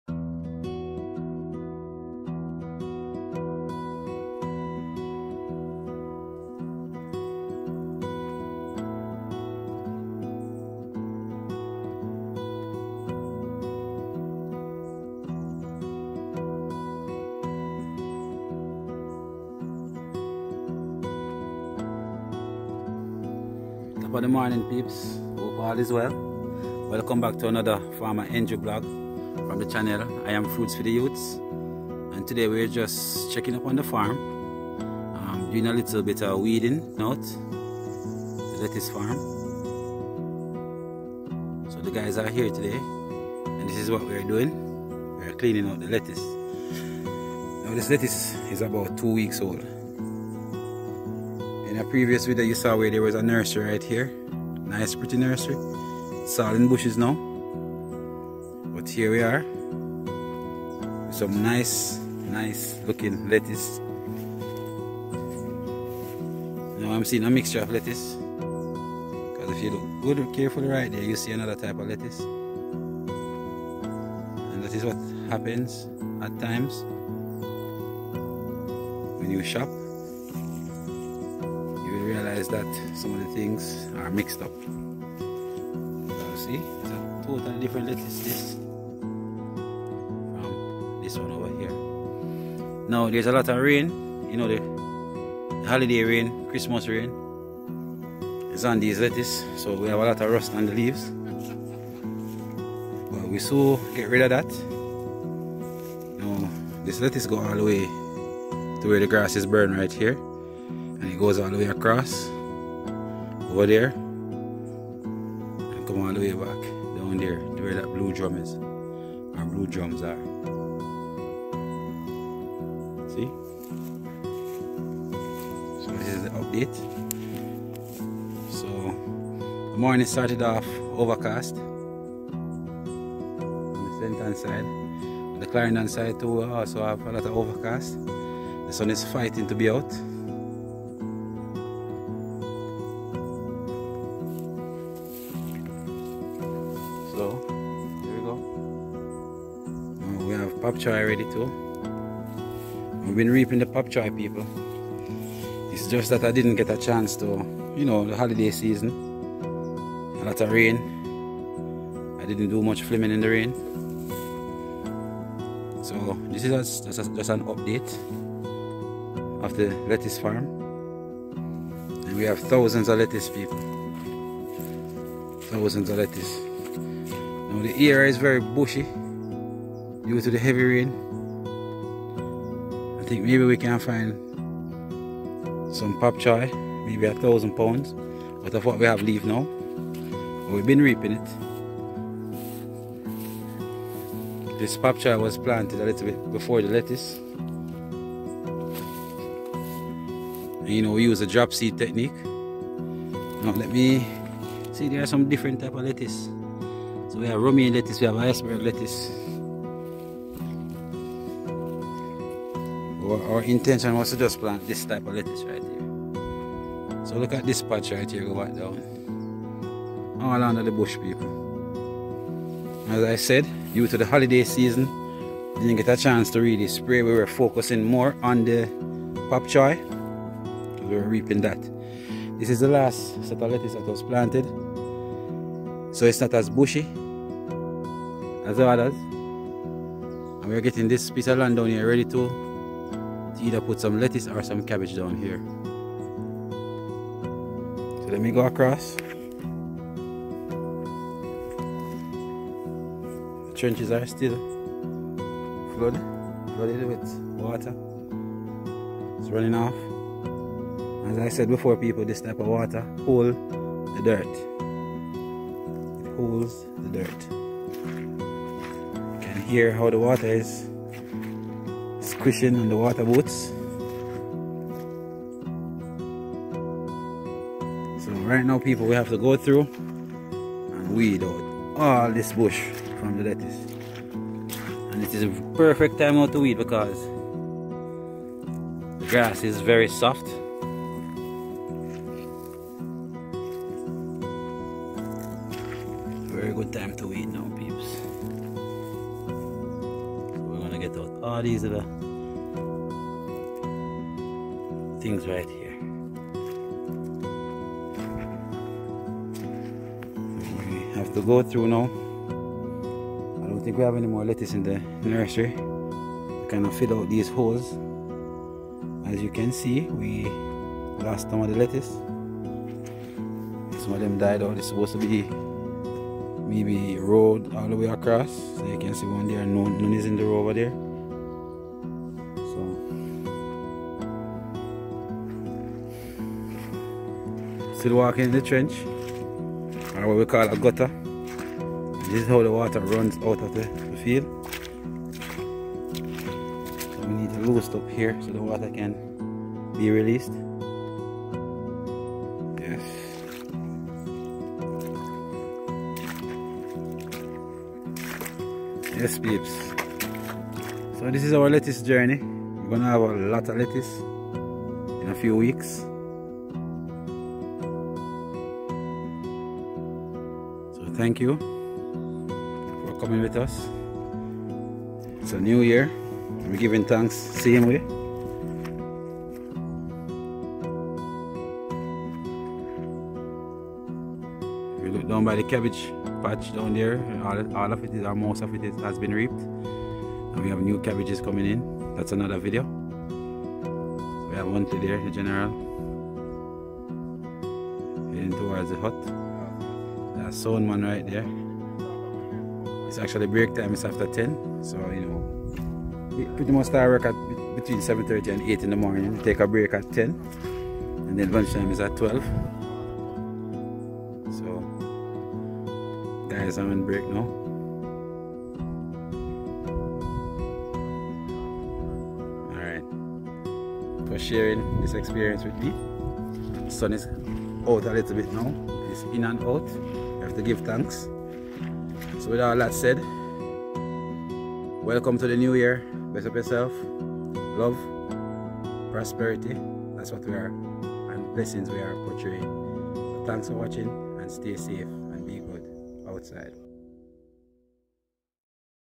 How to groom lettuce in stann jamaica 🇯🇲
In this video we gave you an update of the lettuce on the farm. Hope you found this video very informative.

Come and experience the journey with me as I take you along on the ride❤️💛💚